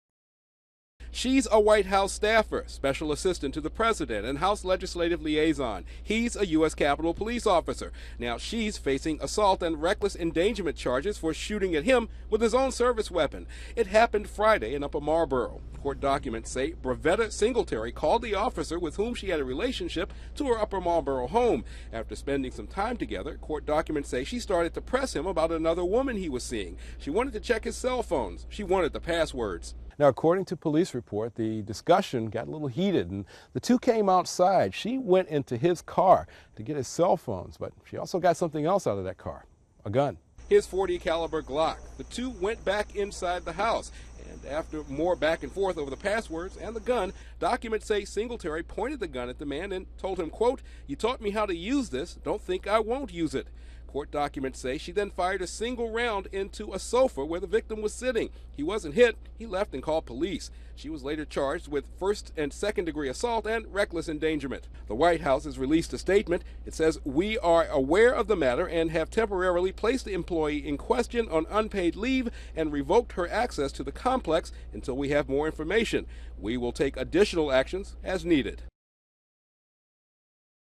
Obama’s Special Assistant Charged With Shooting At Police Officer
A White House staffer was arrested late Friday after allegedly shooting a gun at her boyfriend, an officer with the U.S. Capitol Police, during a heated argument, authorities said.

Barvetta Singletary, 37, who is a special assistant to President Obama and also holds the title of house legislative liaison, was charged with domestic violence in the incident, which police say occurred at her home in Upper Marlboro, Md.

Police say she summoned her boyfriend, who was not identified, by text message, and the two had sex. Afterward, according to the criminal complaint, Singletary went to the man’s car with him and accused him of dating other women. Inside the car, Singletary allegedly took the man’s cellphones and his service weapon, demanding passwords to unlock messages stored on the phones.

“You taught me how to use this,” Singletary allegedly told the man. “Don’t think I won’t use it.”

Police say Singletary pointed the gun in the victim’s direction and fired one round.

The man ran away and called police, according to authorities. Singletary was arrested at the scene without incident.